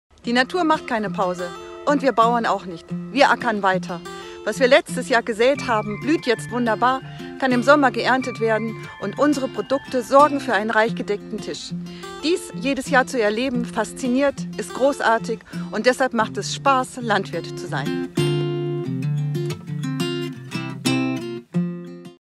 wirmachenweiter SUSANNE
Wir Landwirtinnen und Landwirte arbeiten täglich. Auch in Zeiten der Corona-Pandemie sind wir auf den Feldern und in den Ställen, damit wir alle ausreichend und hochwertige Nahrungsmittel auf dem Tisch haben. Mit Sicherheit.